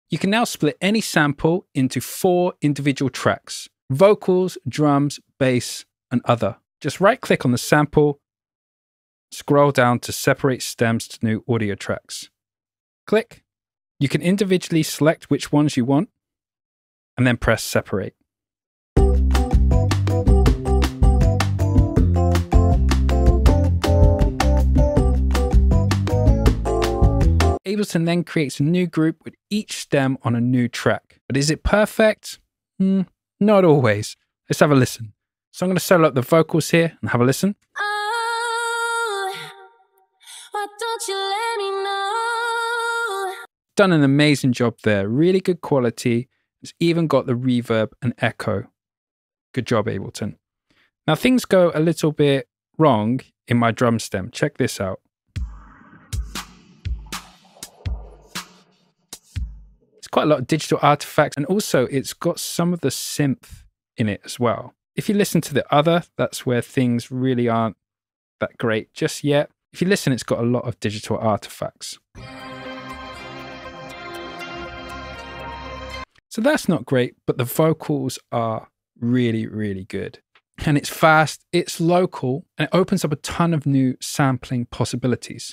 Split Stems Easily with Ableton Live 12.3
Ableton Live 12.3's new feature allows users to separate any sample into four individual tracks: vocals, drums, bass, and other. While the vocal stem separation is impressive, the drum and other stems may contain digital artifacts.